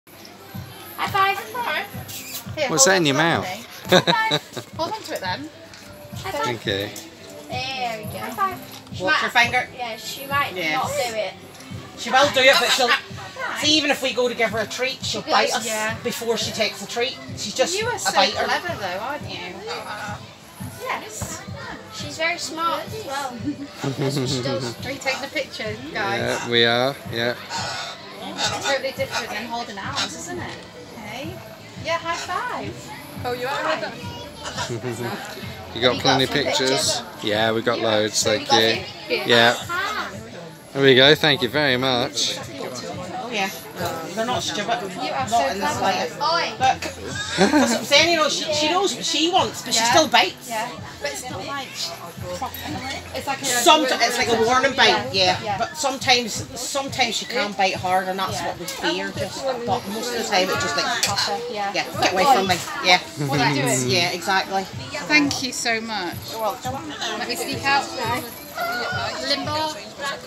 Haven Holidays parrots 2019
Parrot rescue at Haven Holidays Marton Mere 2019.